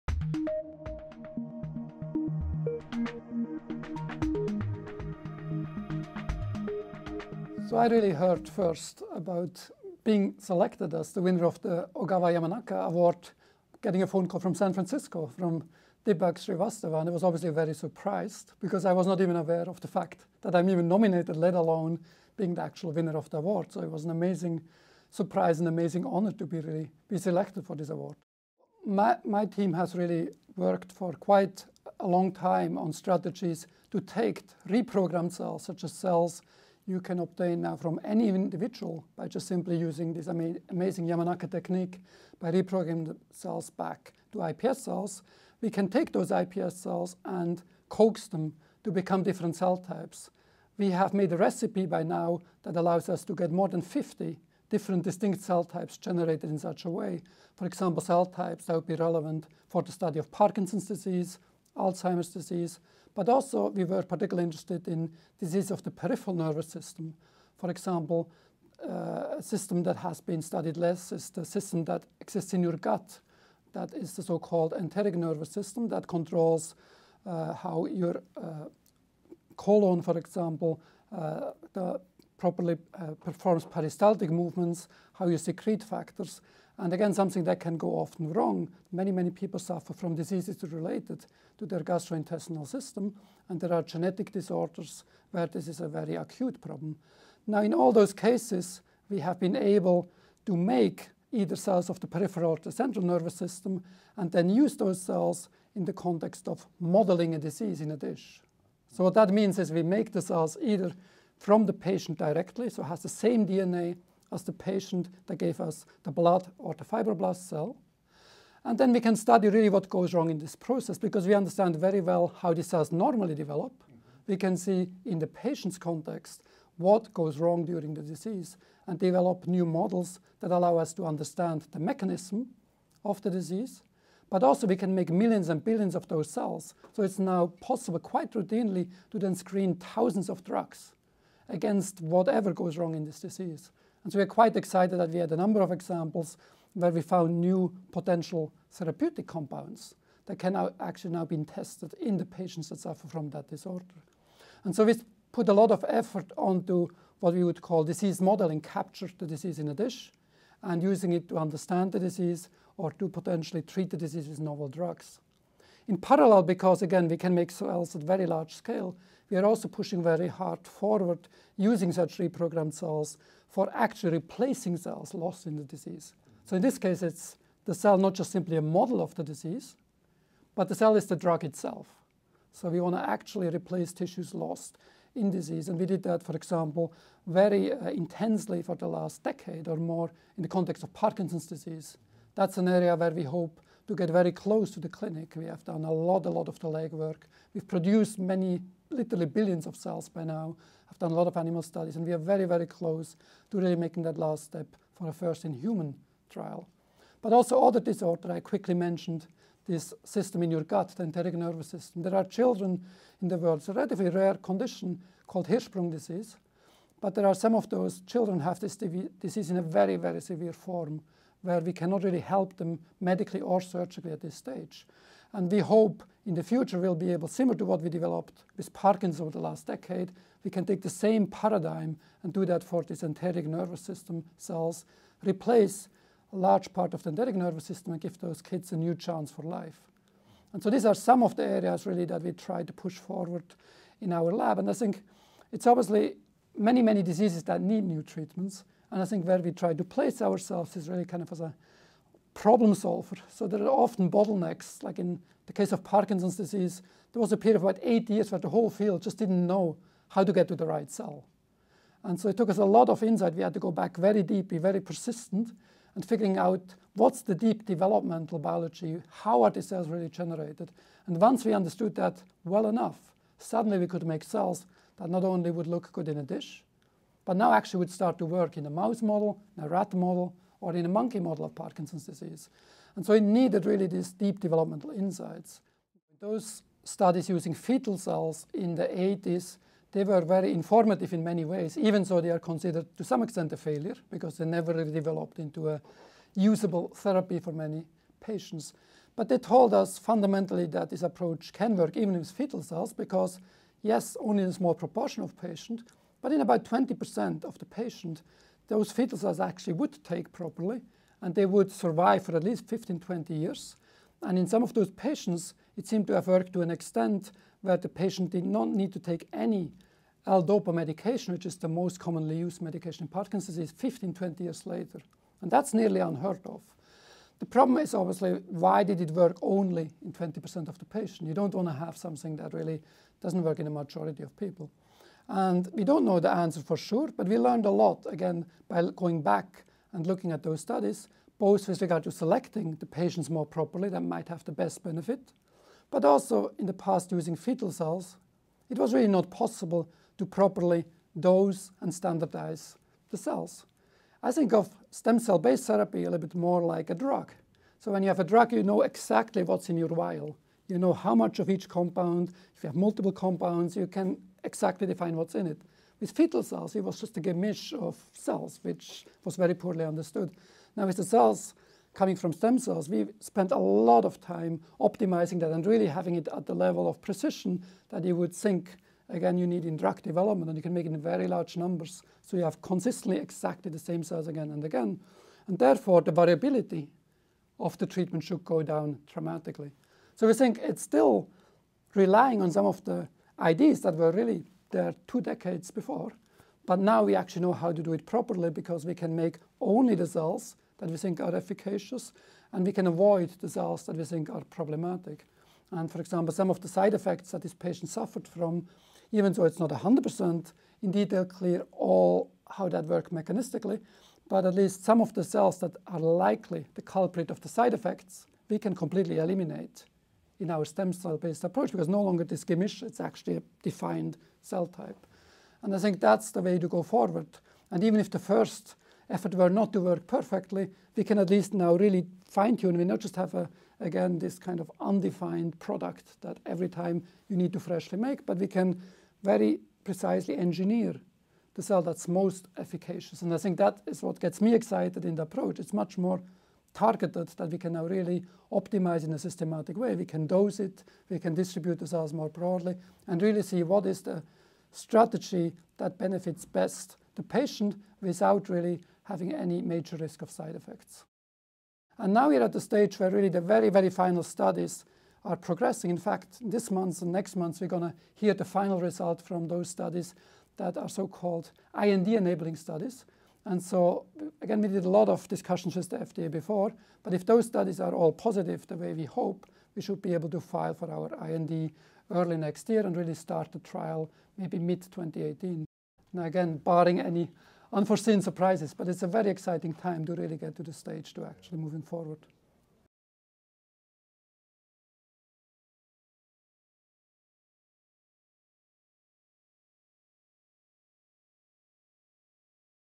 CIRM Interview w Lorenz Studer, 2017 Ogawa-Yamanaka Stem Cell Prize Winner
Don’t miss our video interview with Dr. Lorenz Studor, esteemed stem cell scientist at Sloan Kettering and winner of the 2017 Gladstone Institutes Ogawa-Yamanaka Stem Cell Prize.